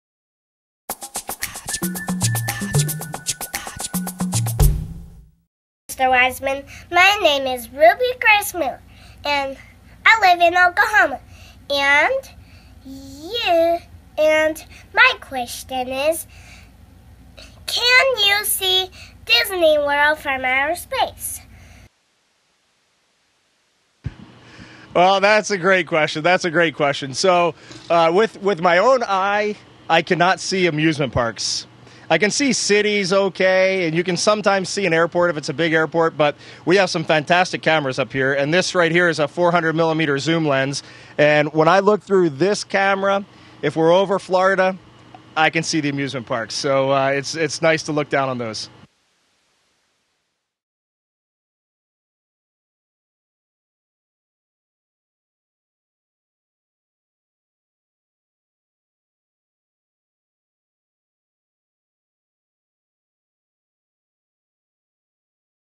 askAstro: Can You See Disneyworld from Space?
Aboard the International Space Station, Expedition 41 Flight Engineer Reid Wiseman of NASA discussed life in space during an in-flight question and answer session Oct. 10. Wiseman answered questions fans submitted on YouTube using askAstro. Questions ranged from aliens in space to the investigations happening onboard to benefit Earth.